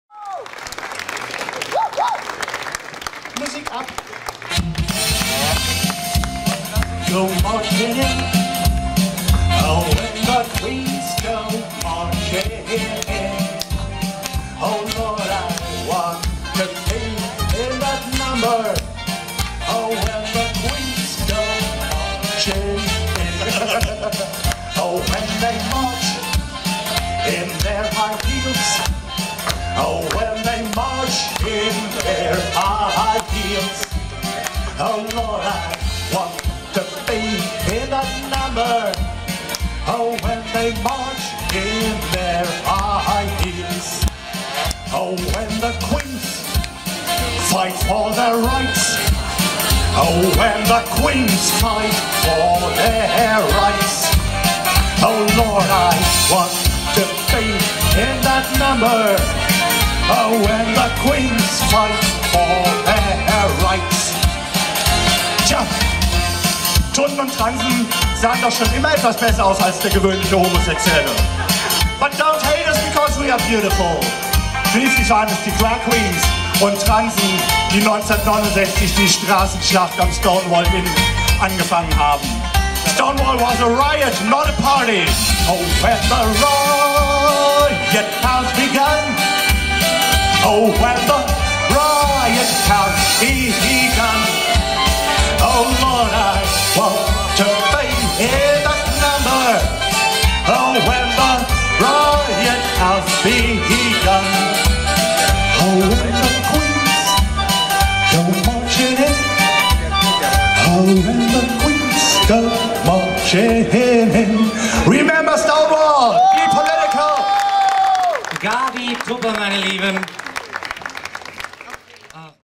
Gaby Tupper auf dem 21.LS Stadtfest 2013
auf der Queeren Medienbühne - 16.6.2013 -
21. lesbisch-schwules Stadtfest